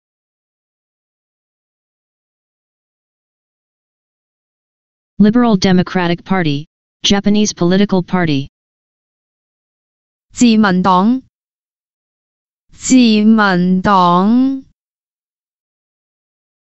"LIBERAL DEMOCRATIC PARTY (JAPANESE POLITICAL PARTY)" in Cantonese (自民黨) - Flashcard
[ji6 man4 dong2] "LIBERAL DEMOCRATIC PARTY (JAPANESE POLITICAL PARTY)" in Cantonese (自民黨) - Flashcard. Additional information about the word: Liberal Democratic Party (Japanese political party).